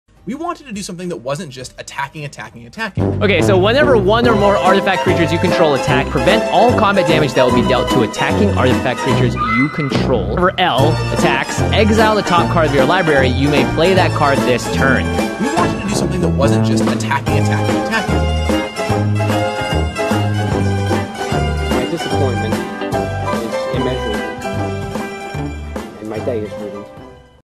Curb your Lorehold
We wanted to make a deck that wasn't based around attacking, so here is 4 new legends and they all care about attacking.